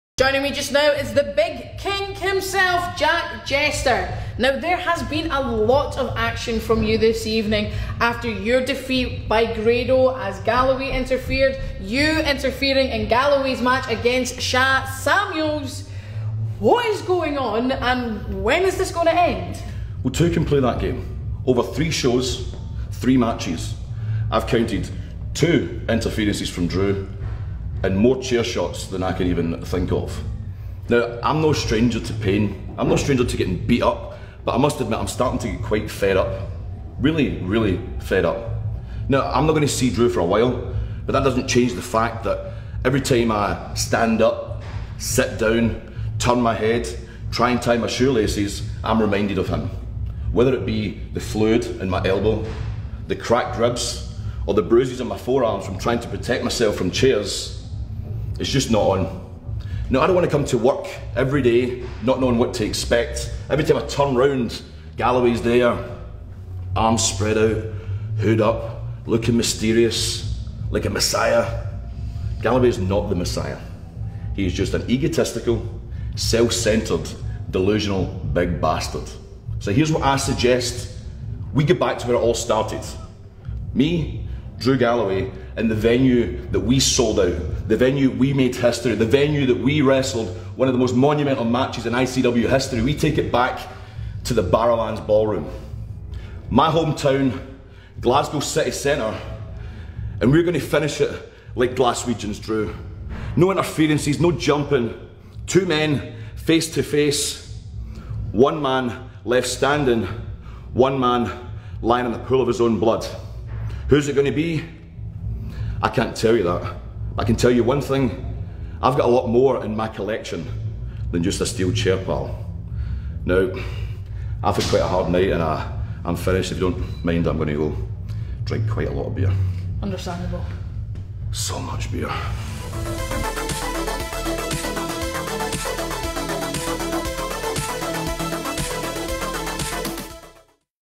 Drew McIntyre Challenged to Final Match
With their recent issues taking over the last the 3 nights of the ICW Fight Club tour, Jack Jester want's to settle things with Drew Galloway once and for all at ICW: Barramania III, on Sunday 16th April at the Barrowland Ballroom!

& Tickets Scotland!